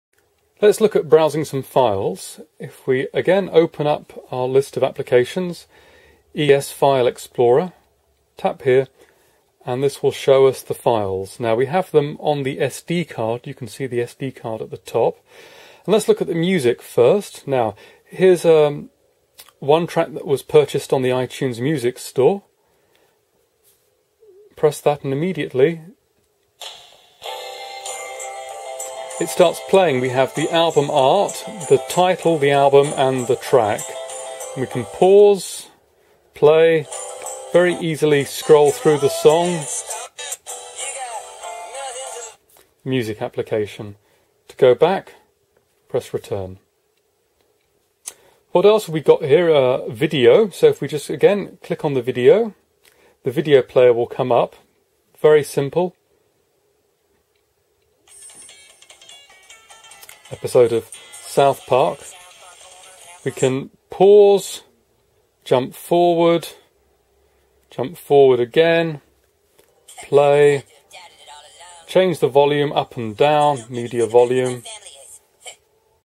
Sonix7 Android Tablet - Music & Movies
New Sonix7 7" Android 2.1 Touch Tablet with WiFi and HD video playback. All your favourite apps and more. Includes HDMI out.